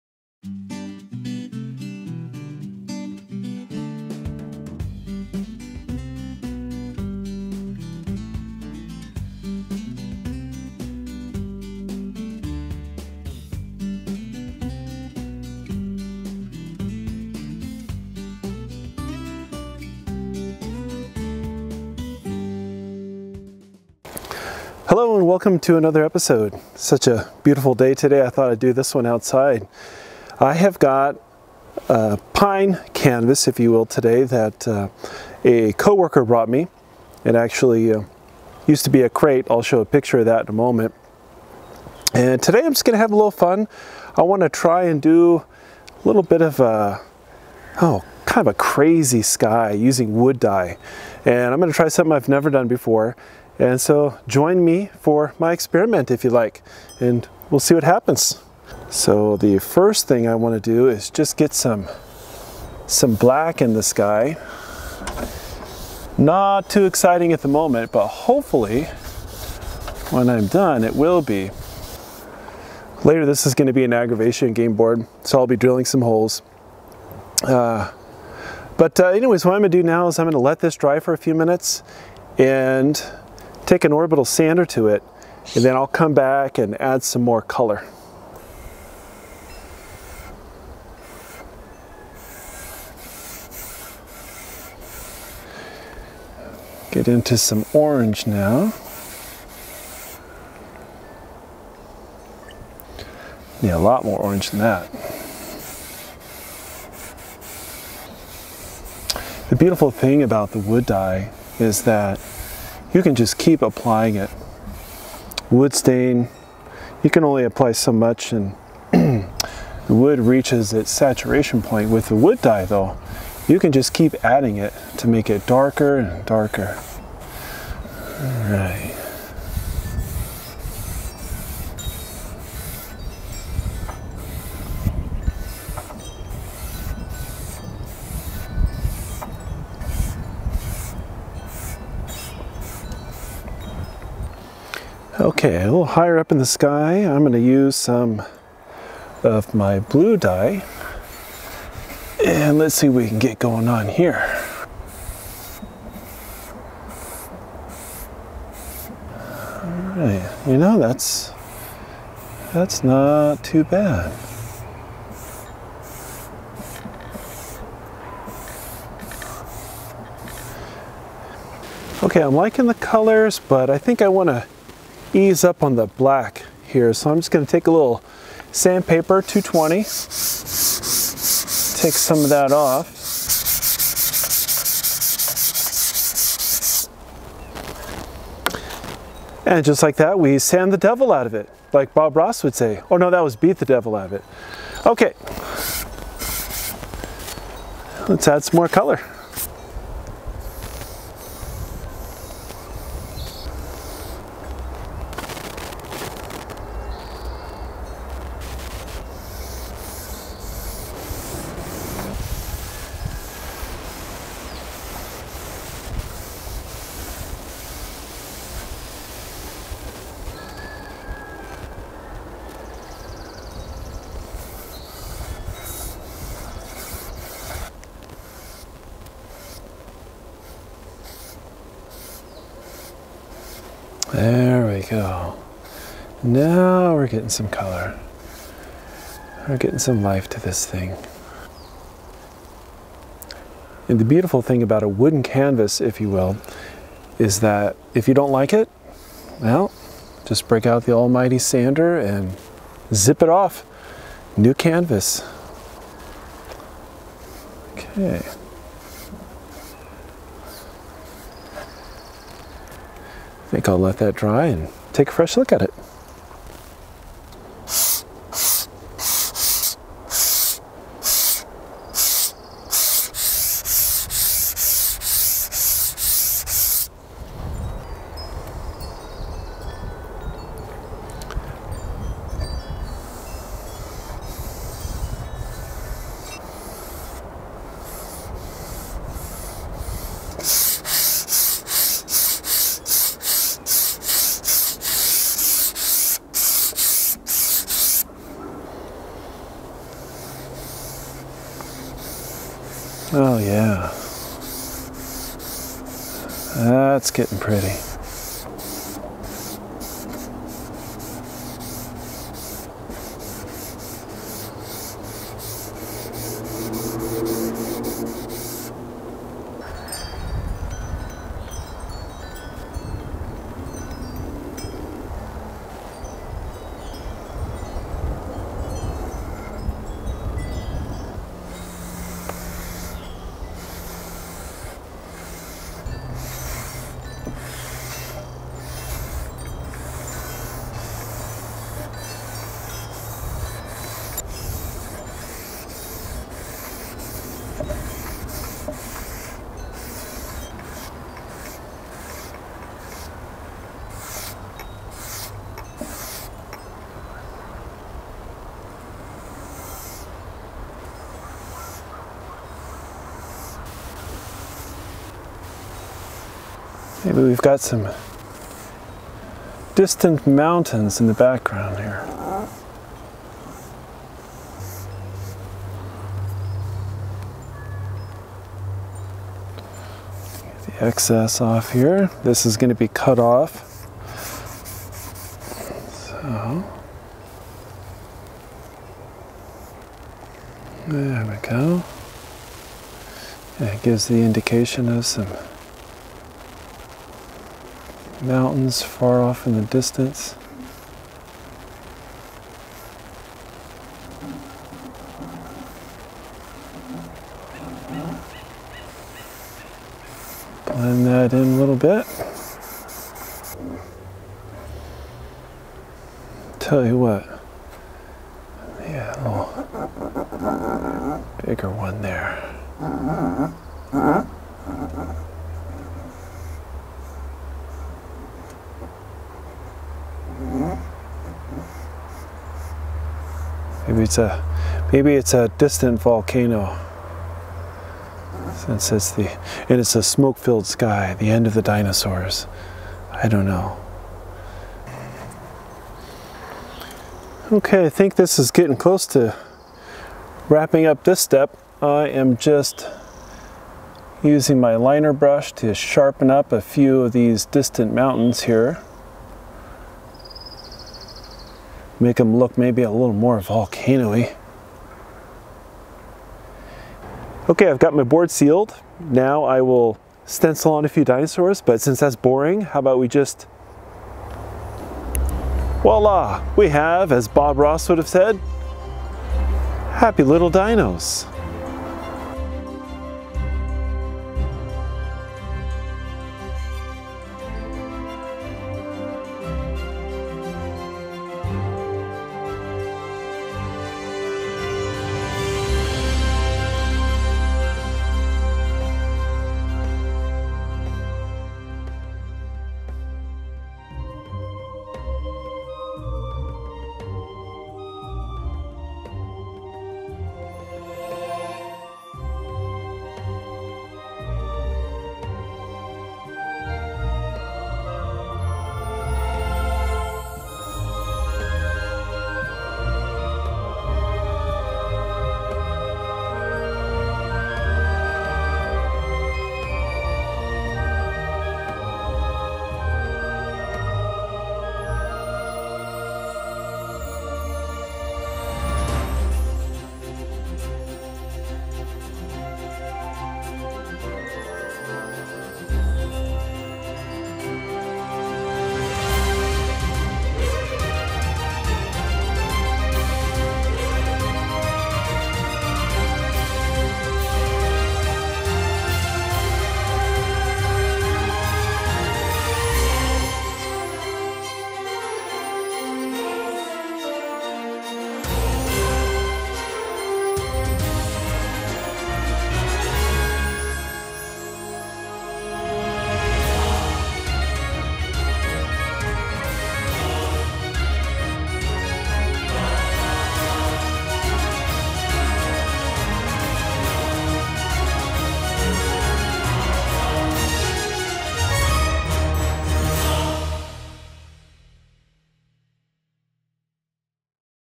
How to Use WOOD DYE to Paint an Ominous Sunset
The project starts off atrocious, but finishes beautifully to my surprise and delight! I demonstrate the brilliant colors you can achieve using wood dye in a craft project. I use Keda wood dye. I use pine wood.